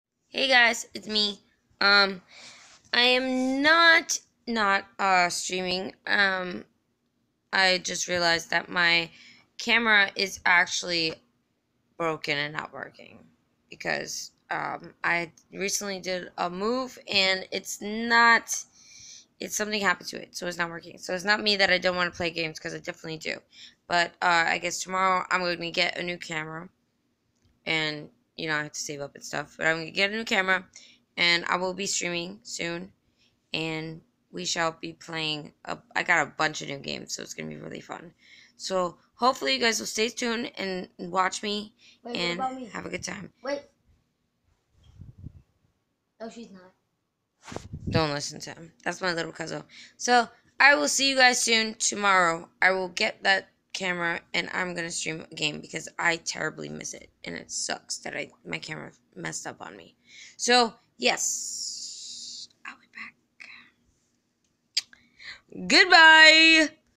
Hello there stranger!
Sorry guys. I've had a set back but I will be playing and streaming tomorrow. It sucks and I am sorry.